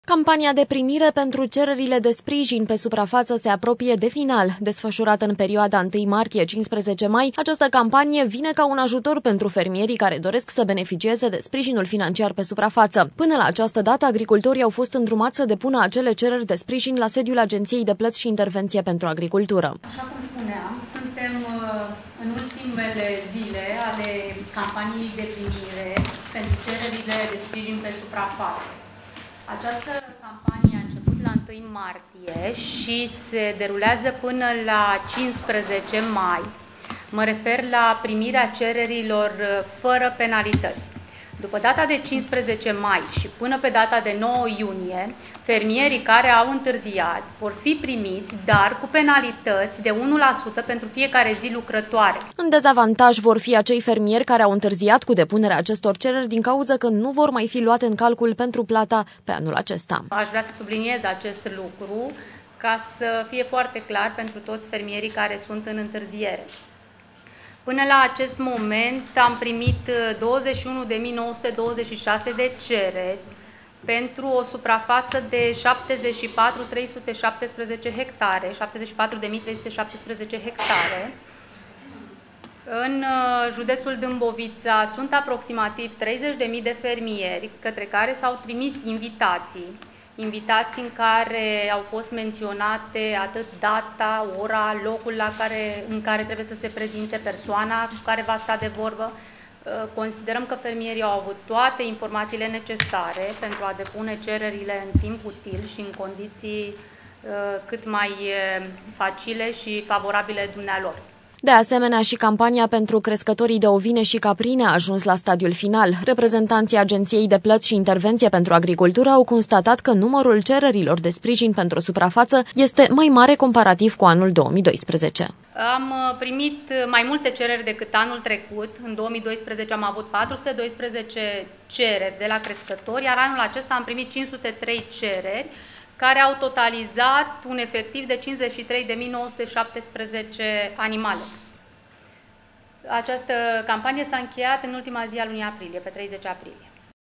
PERIOADĂ ÎNCĂRCATĂ PENTRU FERMIERI (Columna Tv)
Perioada incarcata pentru fermierii damboviteni. In afara de faptul ca acestia trebuie sa aiba grija la modul cum isi ingrijesc recolta in acest sezon, atentia lor trebuie indreptata si asupra campaniei de primire pentru cererile de sprijin pe suprafata desfasurata in intervalul 1 martie - 15 mai. In dezavantaj vor fi acei fermieri care au intarziat depunerea acestor cereri de sprijin, primind penalitati de 1% pentru fiecare zi lucratoare.